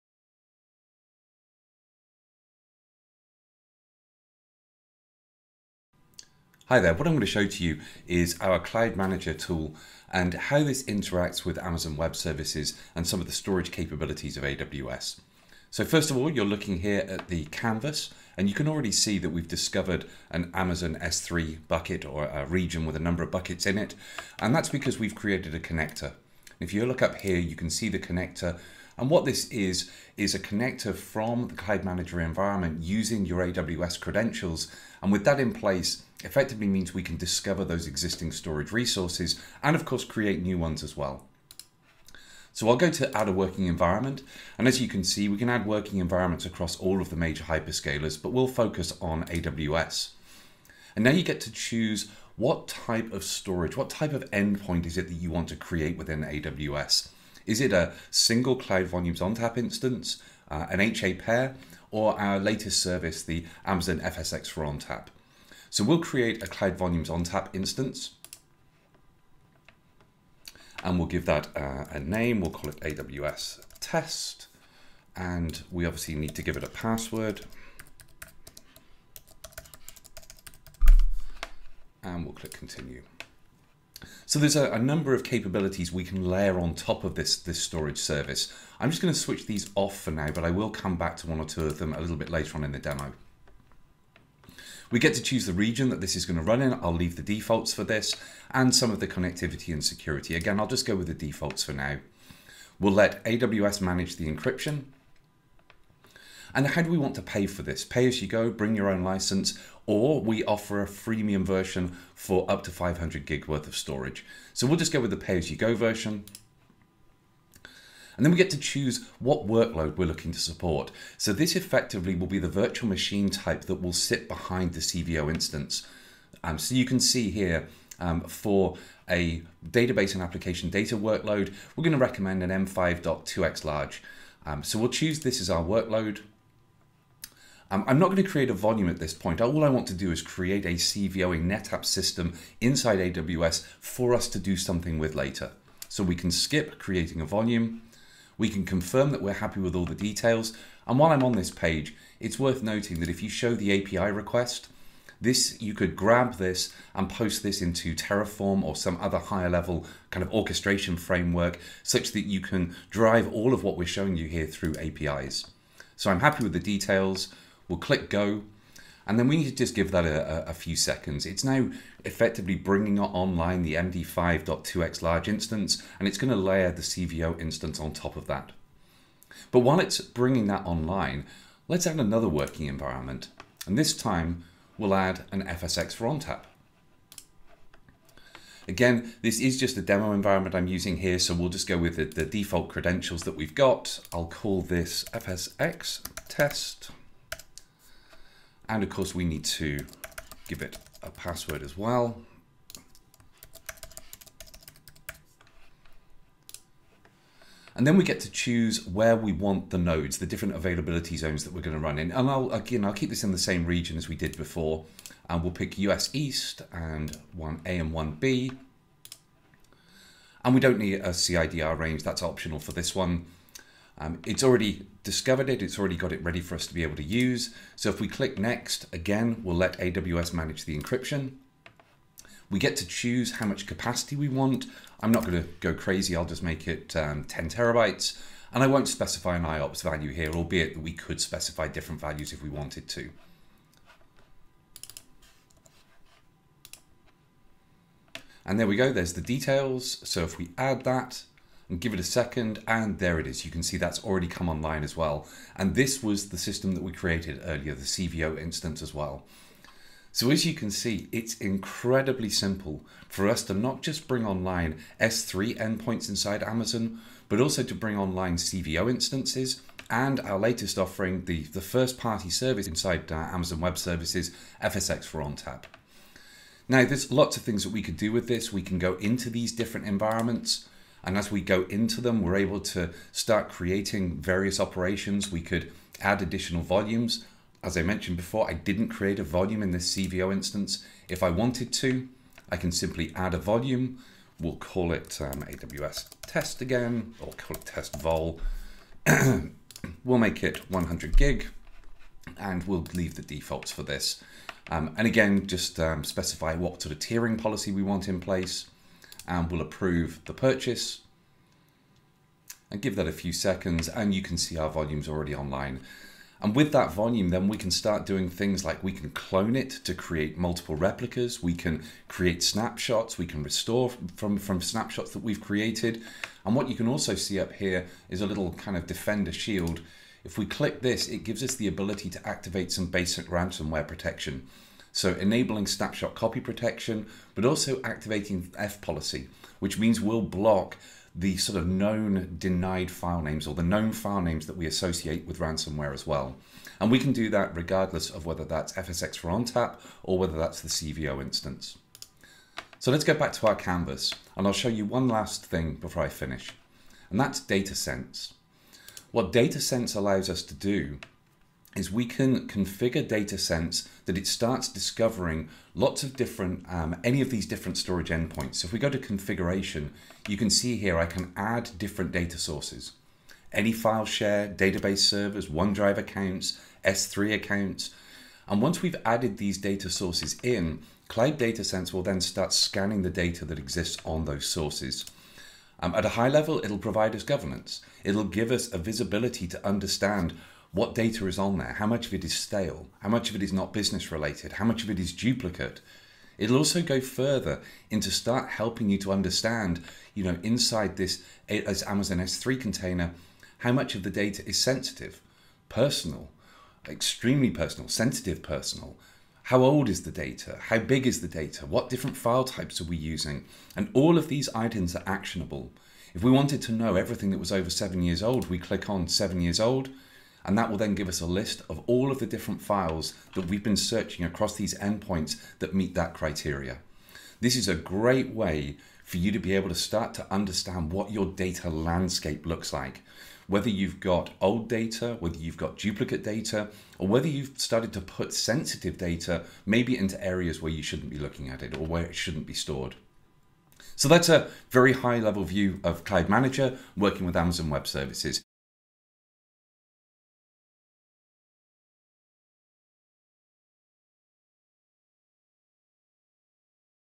NetApp Cloud Manager for Amazon FSx for NetApp ONTAP
Now, ONTAP’s full capabilities are available wherever your data resides with Amazon FSx for NetApp ONTAP, a fully managed, AWS native file service powered by NetApp. NetApp Cloud Manager is a centralized, API-driven control plane to manage your data across the hybrid cloud. Learn how to use Cloud Manager to unlock an ecosystem of tightly integrated NetApp, AWS accessible, and ISV cloud services. Then take advantage of the data protection and management services that made NetApp famous.​